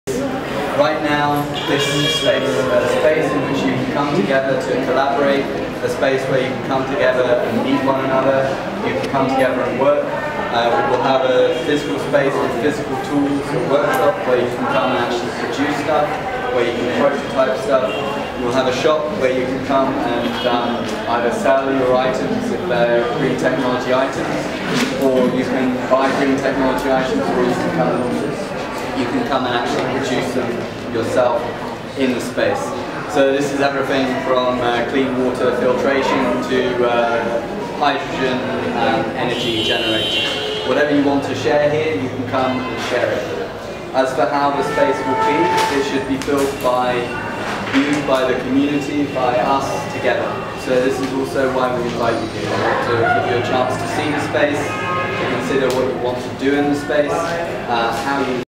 Jay Cousins @icecairo Prelaunch
Jay Cousins speaking during the icecairo prelaunch event.